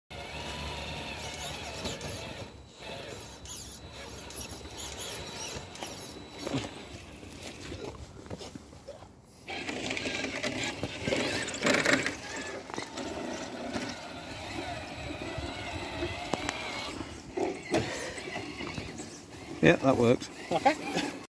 trying out a test sample of a new kinetic winch
kinetic winch test